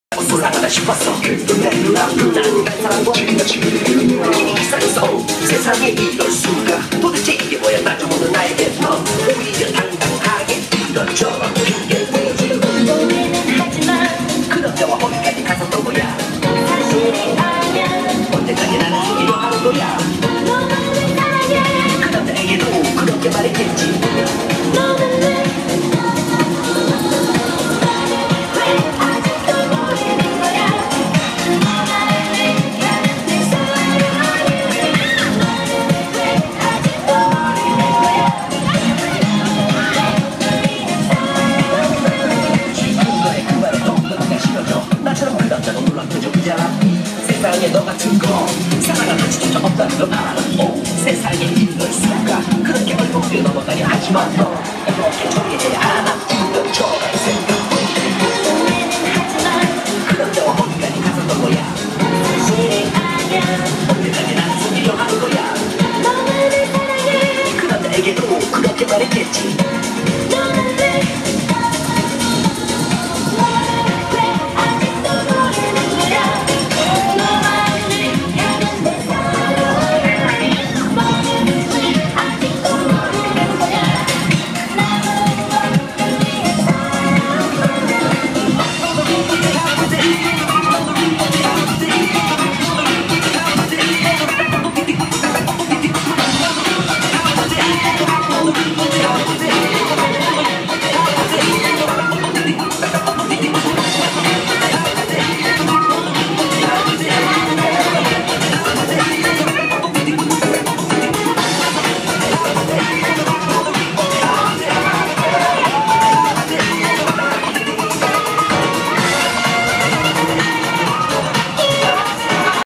월드 디제이 페스티벌에서 디제이 되어보기 체험
지난 18일 월드디젱페스티벌에 참가한 디제이코리아의 이벤트로 취미DJ인 제가 DJ로서 부스에 올라가봤습니다. 생각보다 사람들이 잘 놀아주셔서 고마웠습니다.